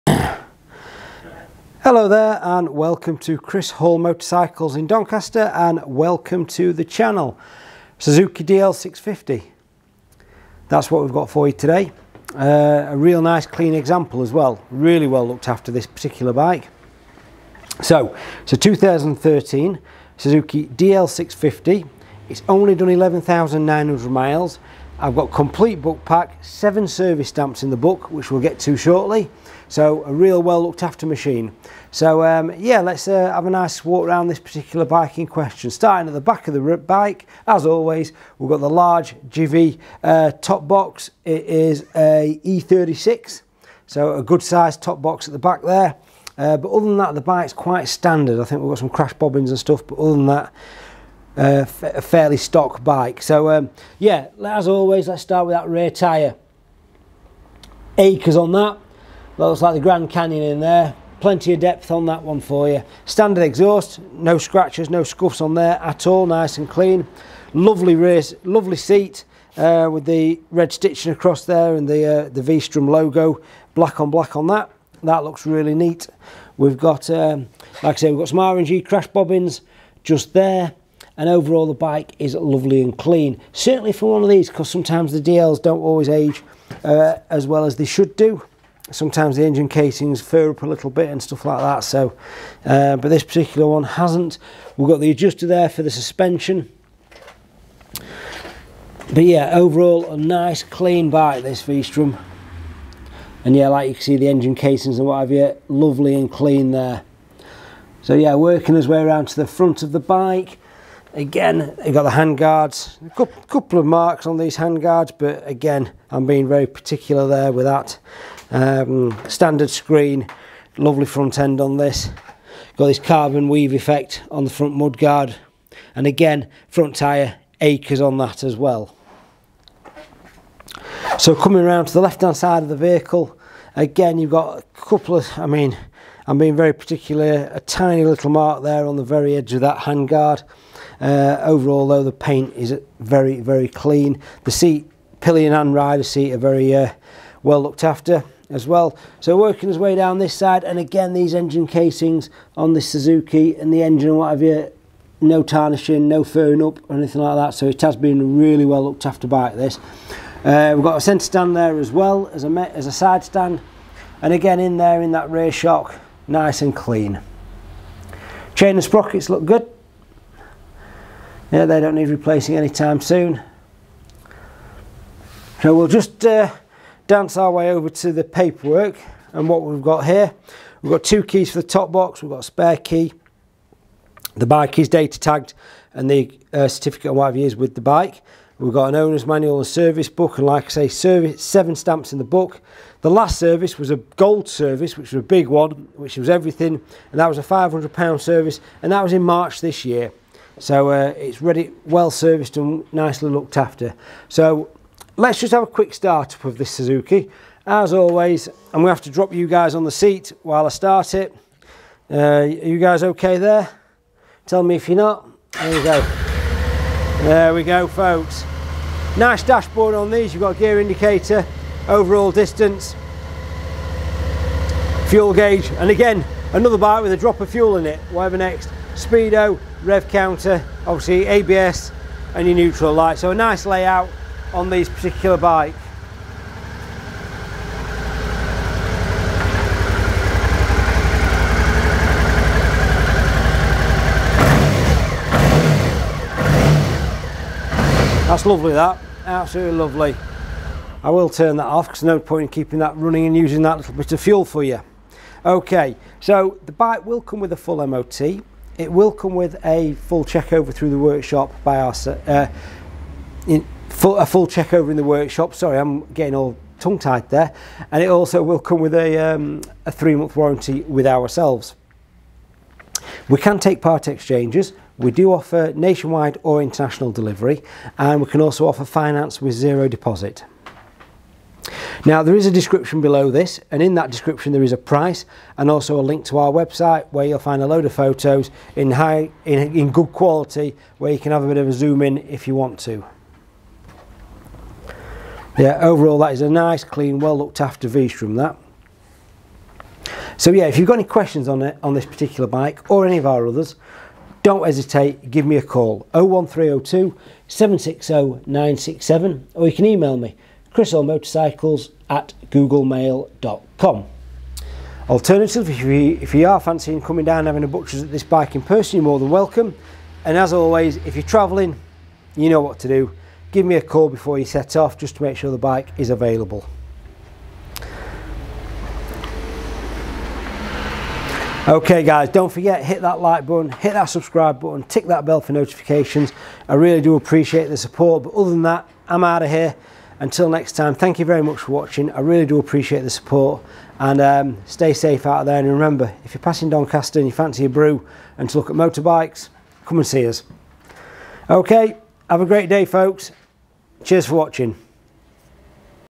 2013 Suzuki V-Strom 650 @chrishallmotorcycles
2013 Suzuki V-Strom 650, a nice clean example of this 650cc adventure bike from Suzuki. This bike has only done 11943 miles from new and has 7 X service stamps in the book. Fitted with large Givi top box, crash bobbins. Supplied with complete book pack including 2 X keys.

SUZUKI V-STROM 650, 650cc , 2013 13 Reg, 11,943 miles , Black 12 month MOT, 3 Month Warranty, ABS, Centre Stand, Service History, Top Box, World wide shipping £4,250.00